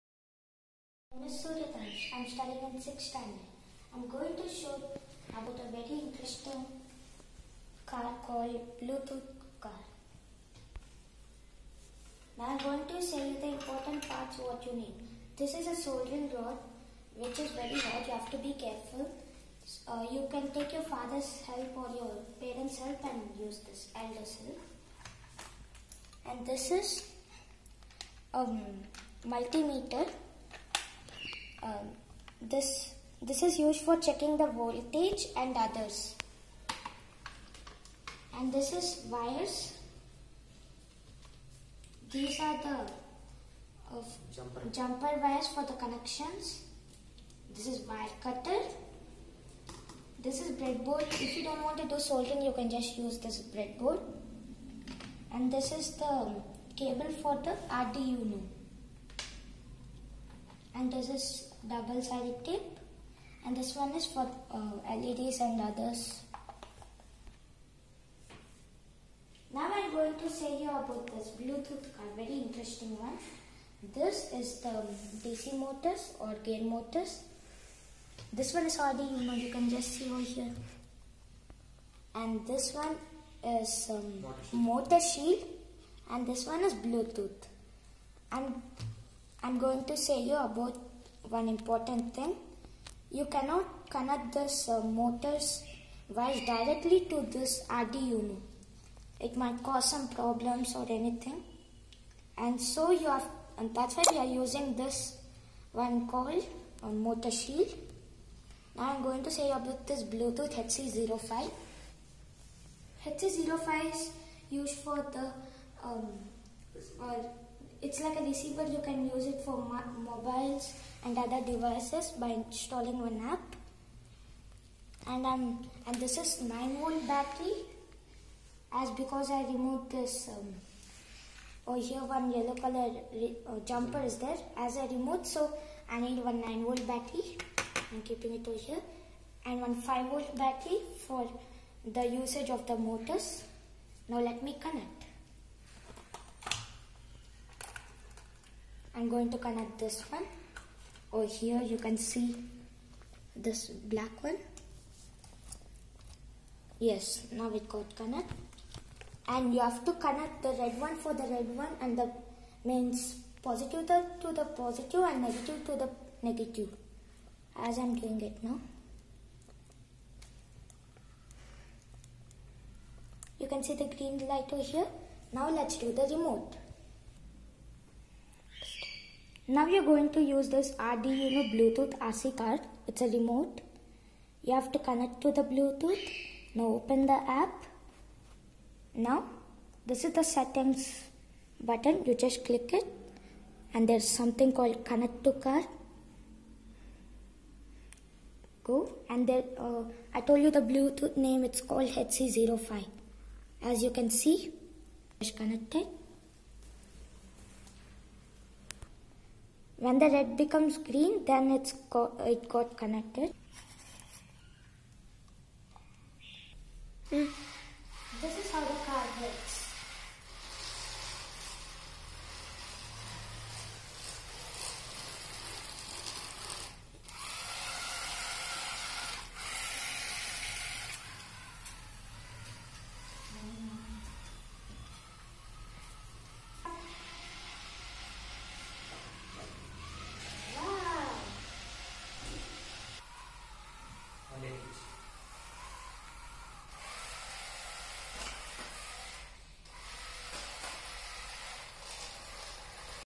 Bluetooth Car Using Arduino and HC-05 By 5th standard student
Added Drift mode.
High Speed mode
and Head and tail lights control.